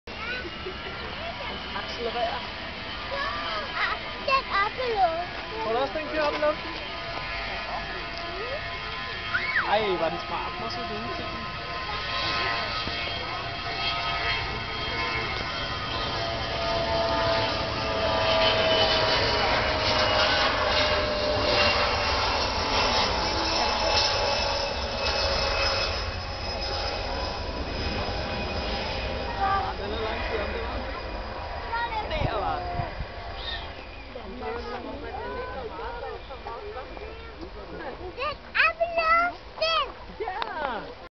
Cimber Air CRJ 200 Take off, Kastrup
Cimber Air CRJ 200 Take off RWY 22R at Kastrup Airport in Copenhagen DK